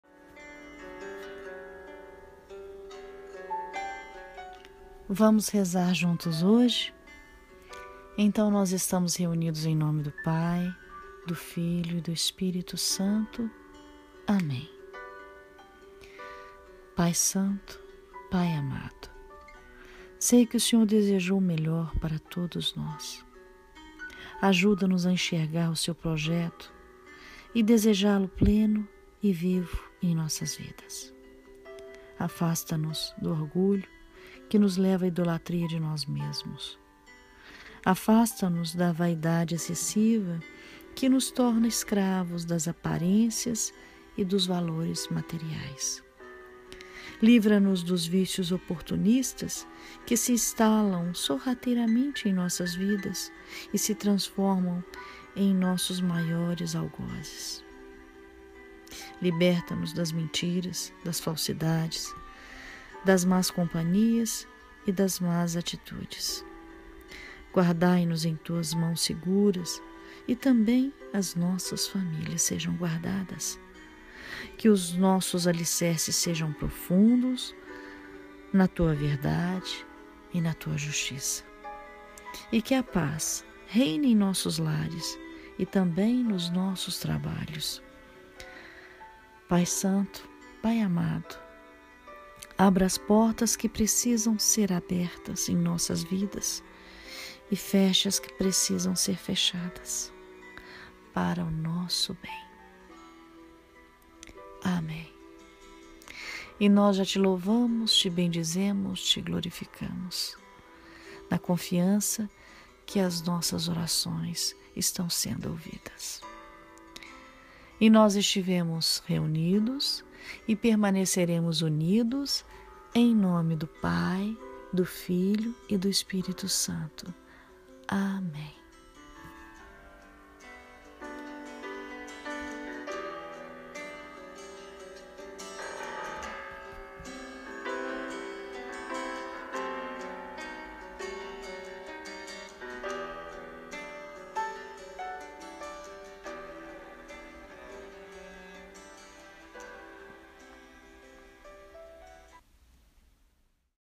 Oração do Dia: 18 de outubro
Uma oração para o seu dia...
Que Deus nos capacite e nos dê entendimento, do que é essencial para nós. Que Jesus feche as portas que precisam ser fechadas em nossas vidas e abra as que precisam ser abertas!